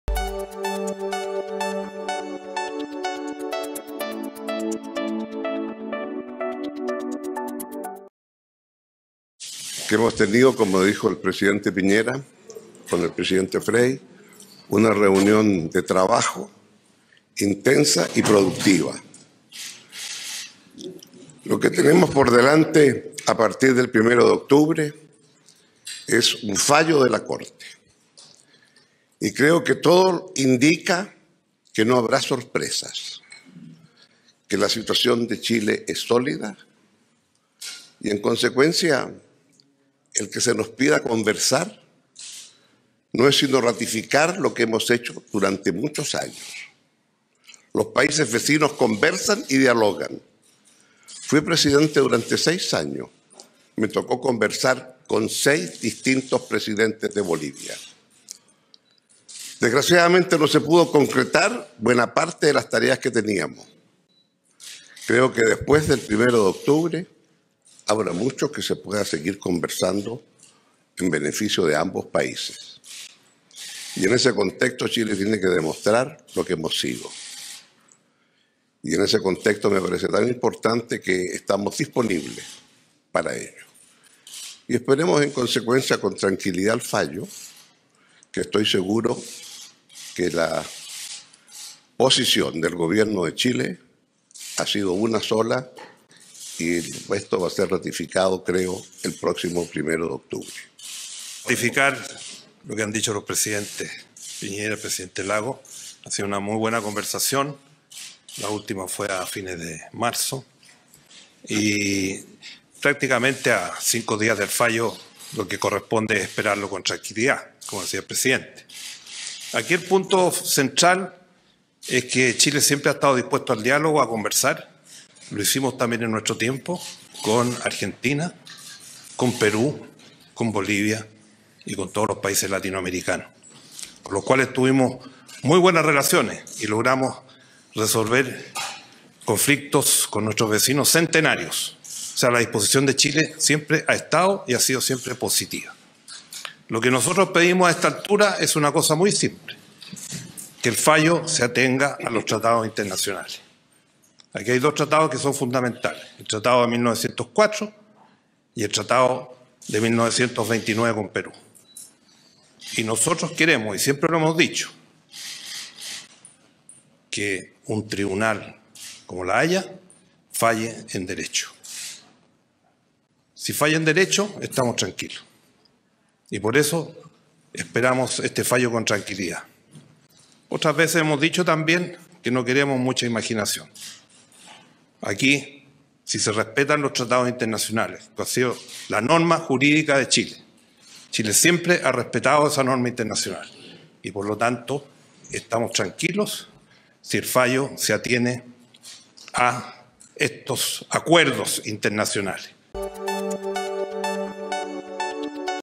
Ahora en DF: Piñera se reúne con expresidentes ante el próximo fallo de La Haya
El presidente, Sebastián Piñera, se reúne con los ex mandatarios, Eduardo Frei y Ricardo Lagos para analizar escenarios ante el próximo fallo del Tribunal Internacional de La Haya que se dará a conocer el lunes 1 de octubre relativo a la demanda boliviana que pide negociar una salida al mar.